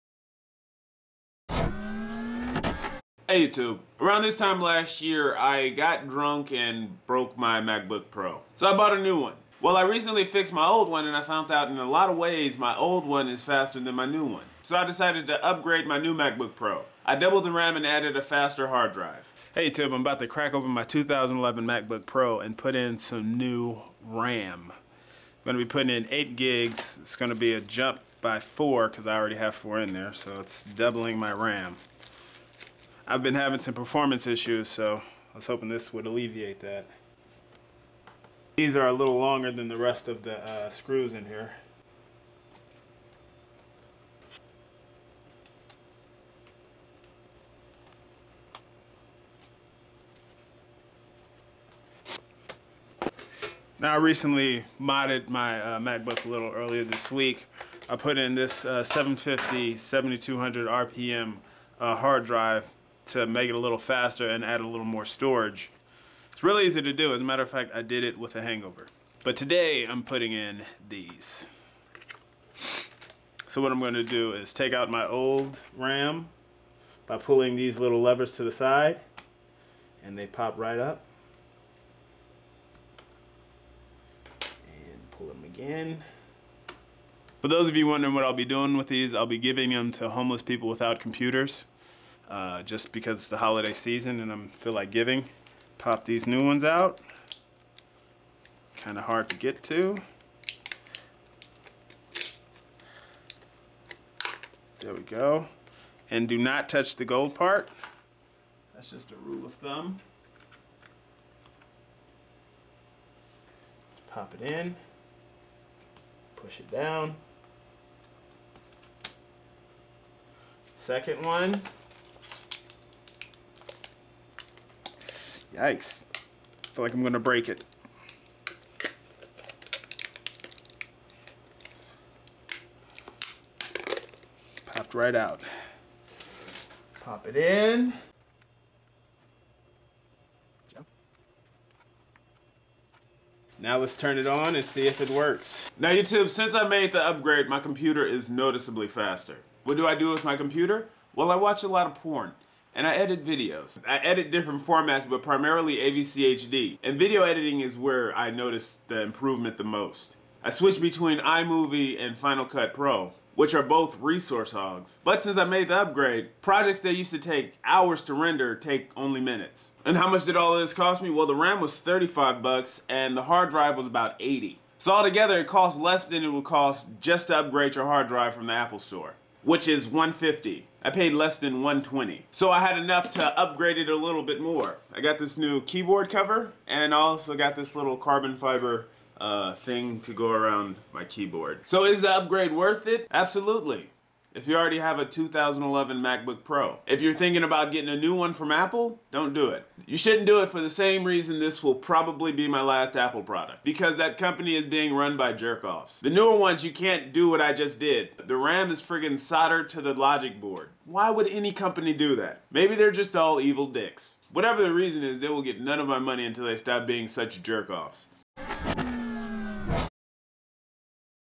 Speed up your Macbook for less that $150
I was able to make my 2011 Macbook pro umpteen times faster for under $150 by adding
8 GB RAM

750 GB HD

NatesVlogs